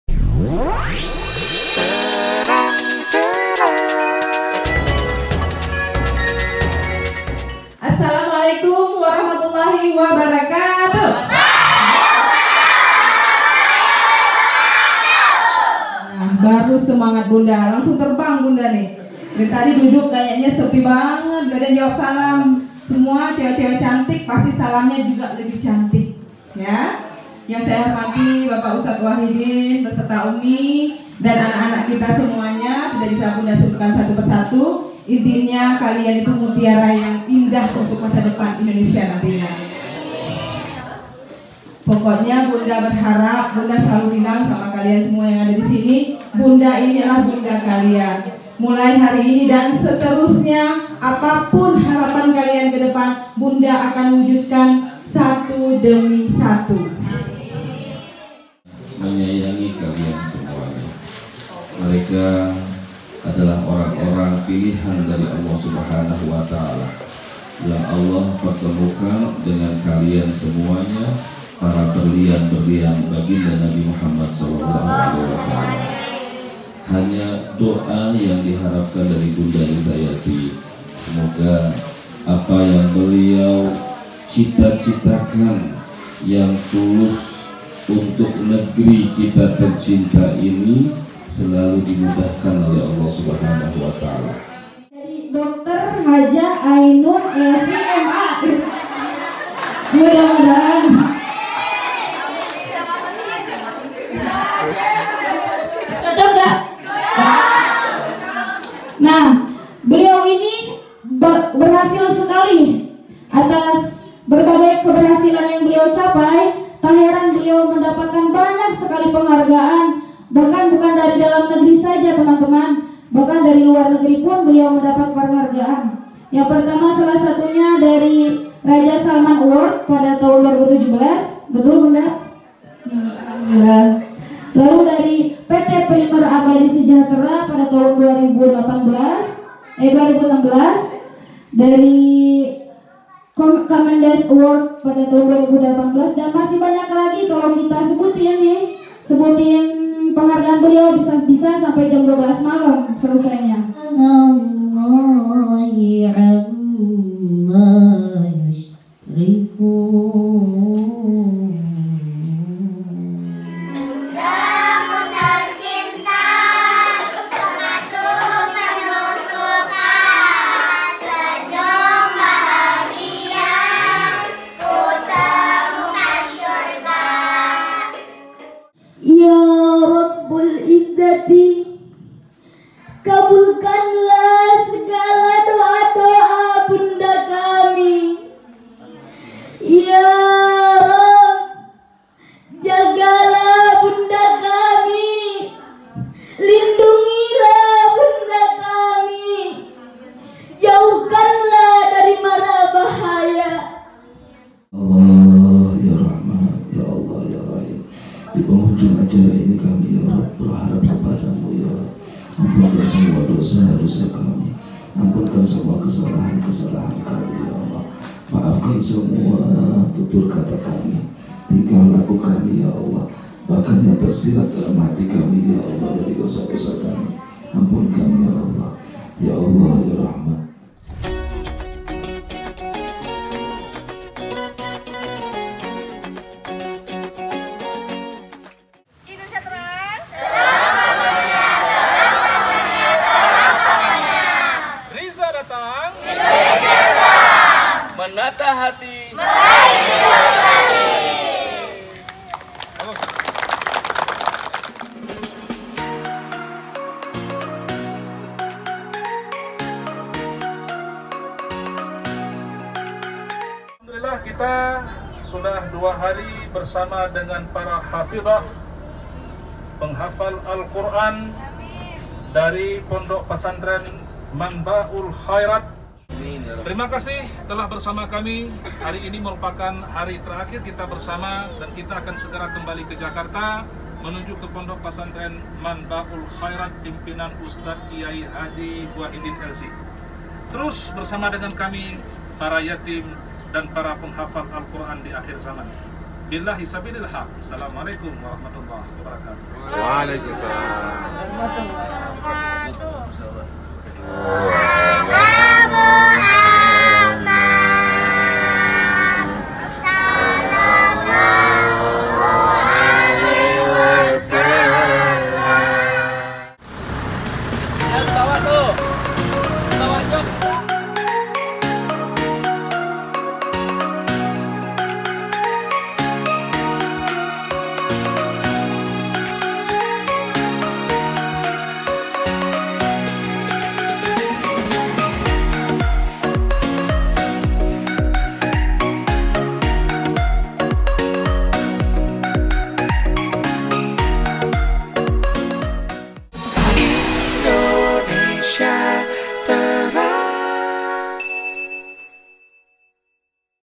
BAHAGIA BERSAMA BUNDA Dr(cn) Hj. Rizayati, S.H., M.M
Santriwati Pondok Pesantren Manba'ul Khairaat Puteri sangat bahagia saat bersama ibunda tercinta.